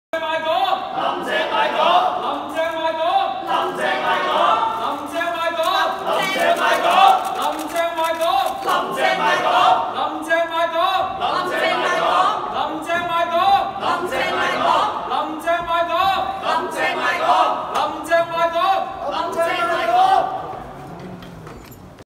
民主派在林鄭月娥質詢時間前示威 20190220
【線報直播】民主派在林鄭月娥質詢時間前示威

行政長官林鄭月娥出席立法會質詢時間，議會陣線在會議廳外請願，批評政府提出修訂逃犯條例。

議員毛孟靜、區諾軒及范國威等手持標語，高叫口號「引渡你返大陸，林鄭賣港」，林鄭月娥進入會議廳時未有任何回應。